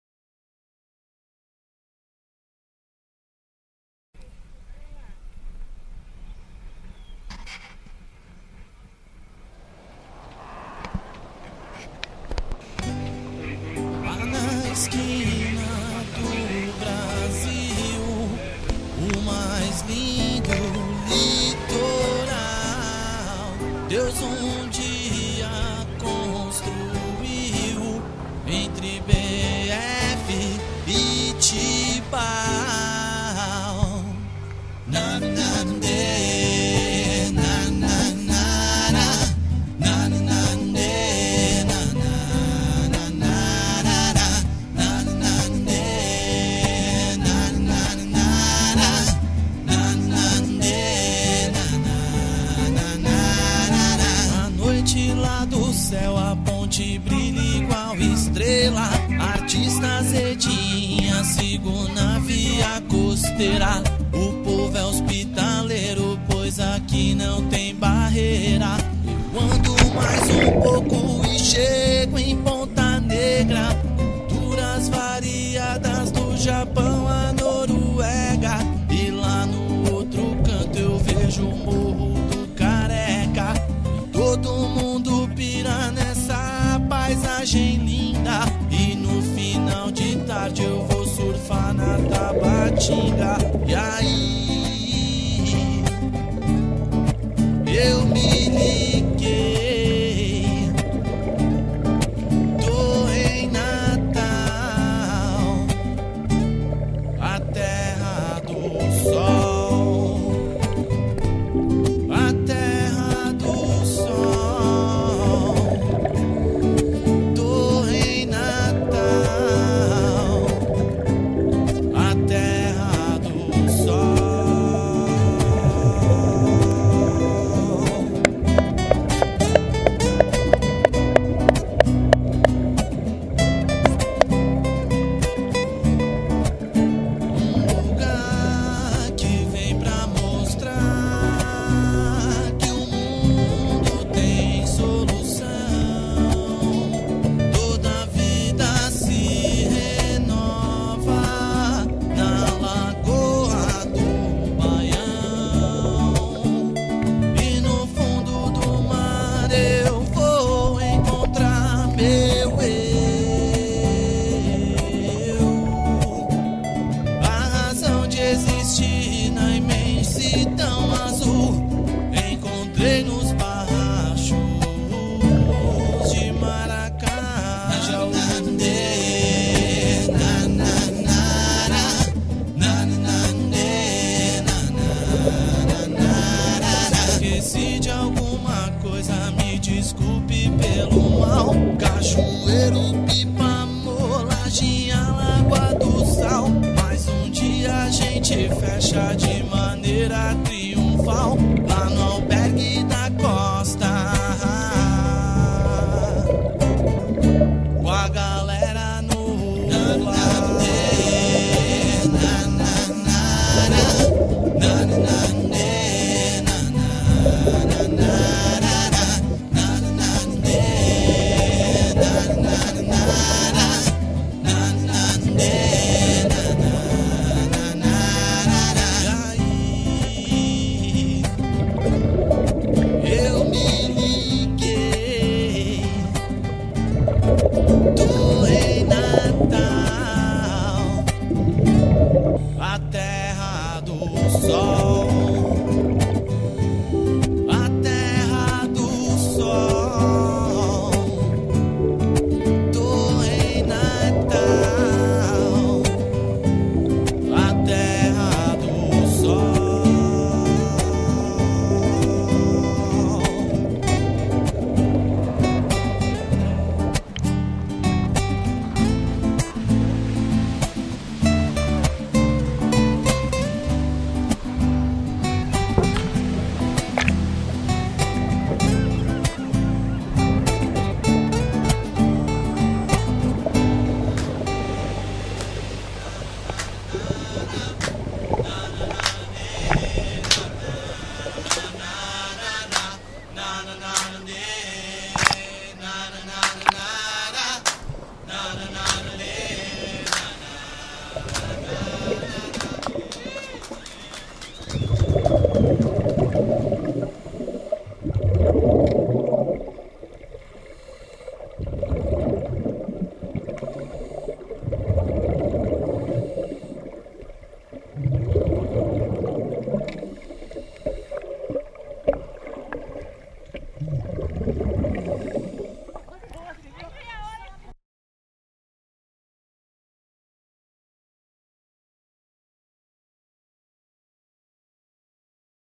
Maracajau mergulho video do dia 24-12
Mergulho de natal, preservemos o médio ambiente não pise os corais, faz parte não destruía, preservar e viver.